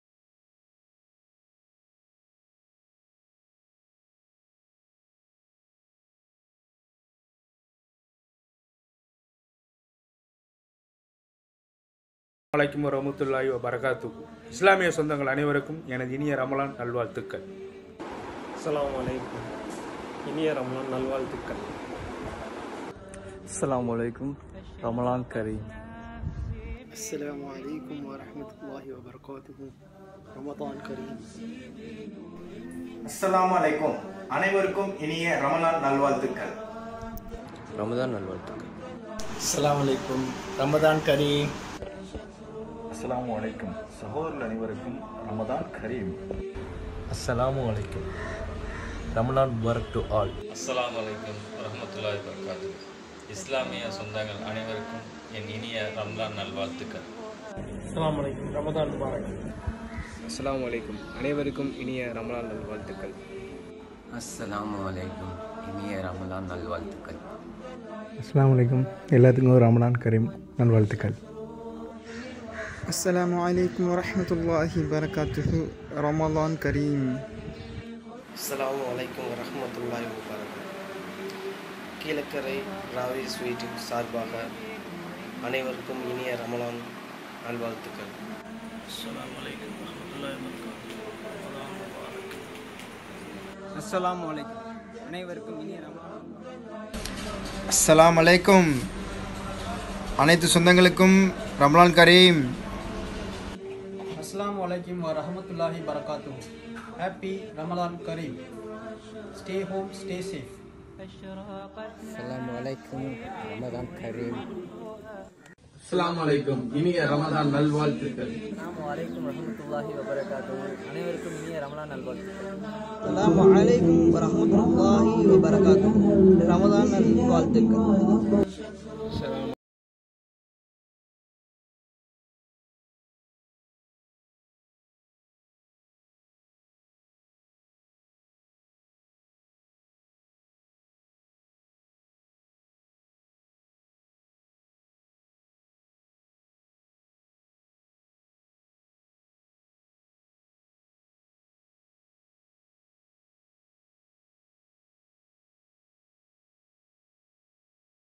RAMADHAN WISHES | Stylish keelakarai | Ramadhan 2020
வெவ்வேறு ஊர்களில் நாடுகளில் வசிக்கும் கீழக்கரை மக்களிடம் இருந்து ரமலான் வாழ்த்துக்கள் இதோ